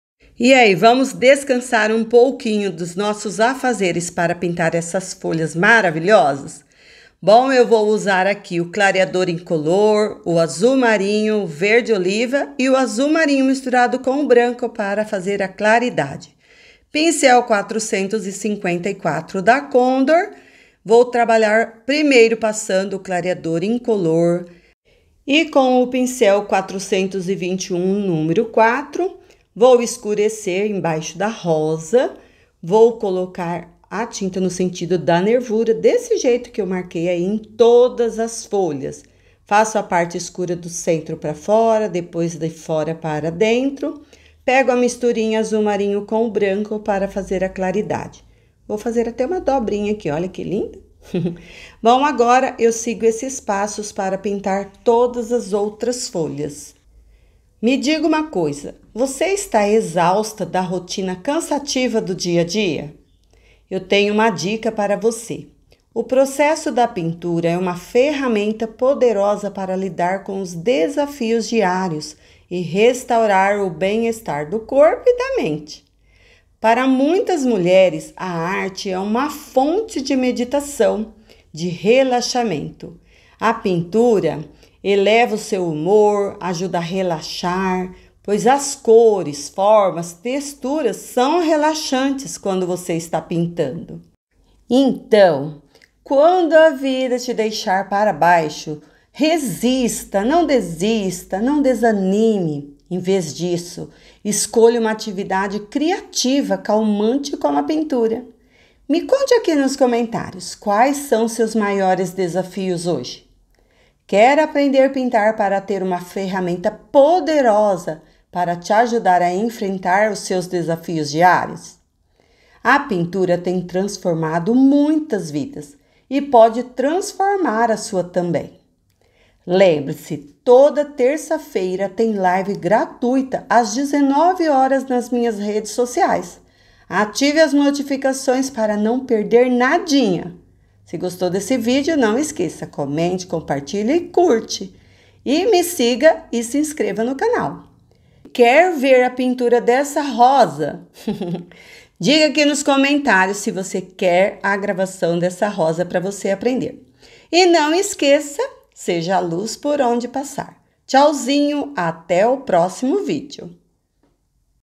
Está exausta da rotina cansativa do dia a dia!
Eu tenho uma dica para você!
O processo de criação artística é uma ferramenta poderosa para lidar com os desafios diários e restaurar o bem-estar para o corpo e a mente.

Para muitas mulheres, a arte é uma forma de meditação. A pintura ou o desenho elevam seu humor e o ajudam a relaxar com as cores, formas e texturas relaxantes.

Quando a vida te deixa para baixo, resista não desanime! em vez disso, escolha uma atividade criativa calmante, como a pintura!

Me conta aqui nos comentários quais seus maiores desafios hoje?
Quer aprender pintar para ter uma ferramenta poderosa para te ajudar a enfrentar seus desafios?!

A pintura tem transformado muitas vidas e pode transformar a sua também.

Quer ver a pintura dessa rosa diga nos comentários
Não esqueça seja luz por onde passar!